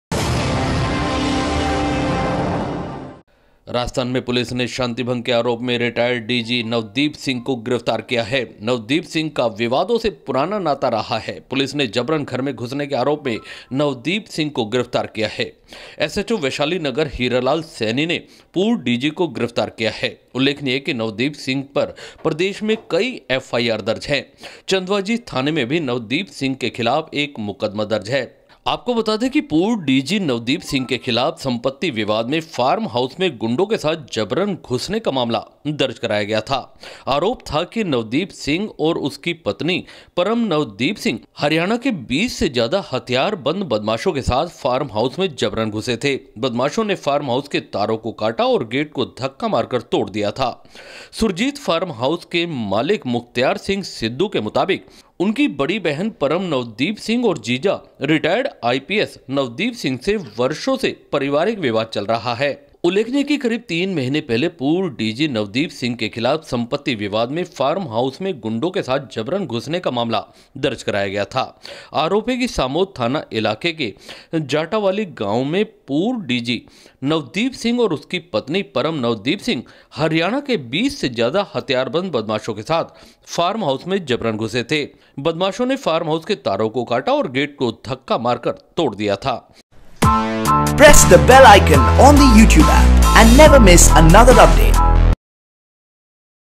डीजी नवदीप सिंह क्यो हुए गिरफ्तार.
प्रोपर्टी विवाद के चलते फार्म हाउस पर हुआ विवाद.
उनके साले ने हरियाणा के 20 गुंडों सहित नवदीप सिंह पर लगाये जबरन घुसकर मारपीट के.